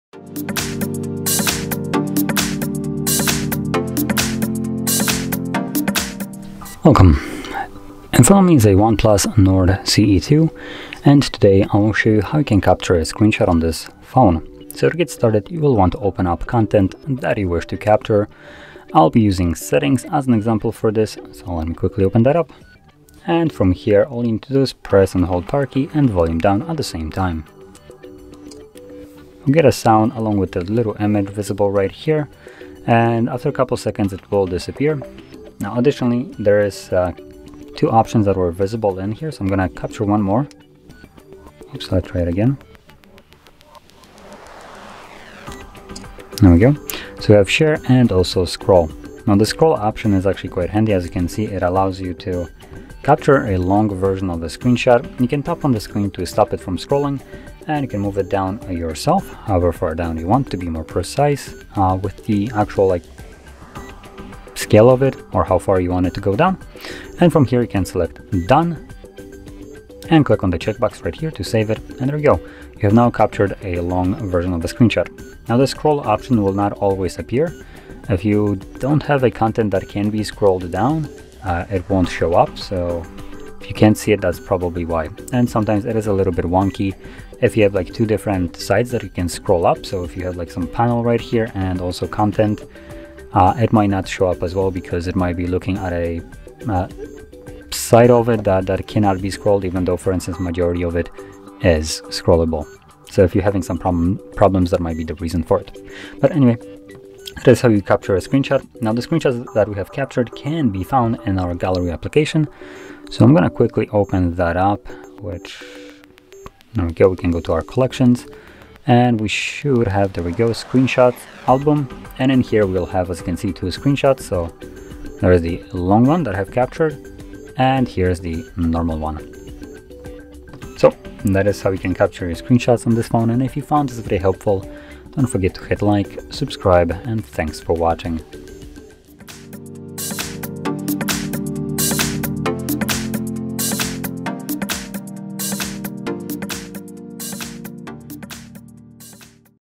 How to Capture Screen of OnePlus Nord CE 2 - Take Screenshot
In this tutorial, we’ll love to show you how to Capture the Screen of the OnePlus Nord CE 2! If you wish to take Screenshots within seconds, whenever you want to, then this tutorial is just perfect for you. Our expert will show you the most efficient way of taking screenshots within seconds. So, without further ado, grab your One Plus, and let’s get started!